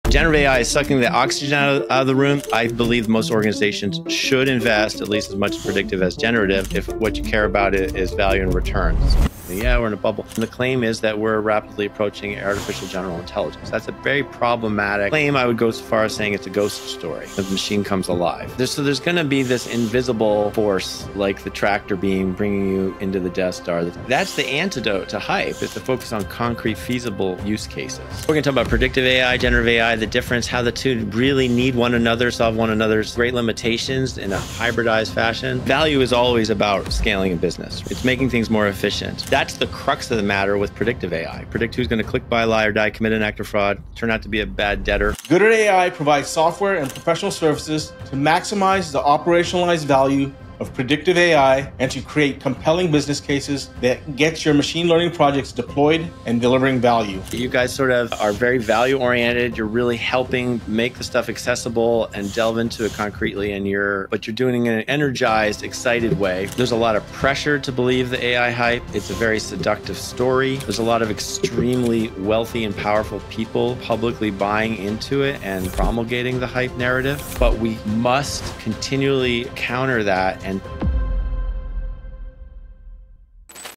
Trailer | Balancing Predictive & Gen AI for Real ROI | Eric Siegel | Founder & CEO | Gooder AI | 4K
Eric Siegel, Founder and CEO, of Gooder AI explains to Sanjay Kalluvilayil, Founder and CEO, of Stonehaas Advisors the difference between Predictive AI and Generative AI and how the two really need to work to solve one another's great limitations in a hybridized fashion. How can we begin realizing the bold promise of generative AI autonomy by introducing a new kind of reliability layer that is semi autonomous, adaptive, and built with bespoke guardrails tailored to each specific problem we aim to solve? They try to put the AGI hype in its place and differentiate between the real feasible potential value and use cases versus a lot of the public narrative that tends to over promise.

Key Insights
1. Machine Learning and Predictive AI still have untapped opportunities to drive real business ROI, but is being overshadowed by generative hype.
2. Generative AI needs a “reliability layer” with guardrails and 'humans in the loop' to be production-ready and ensure quality,
3. The future of AI is hybrid, combining predictive precision with generative flexibility to generate meaningful results.
4. The "AGI" and “agentic AI” hype cycle risks creating an unsustainable AI bubble with severe consequences
5. Real AI success comes from human collaboration, grounded strategy, and patience to find the best places to grow and scale an organization.

🎙️ The AI Space Podcast — Ep. 24  | Trailer
Guest: Eric Siegel | Founder & CEO | Gooder AI

AI, Artificial Intelligence, AI Podcast, AI News, Tech News, GPT

Join the AI Revolution - Artificial Intelligence Insights for AI Startups & Founders!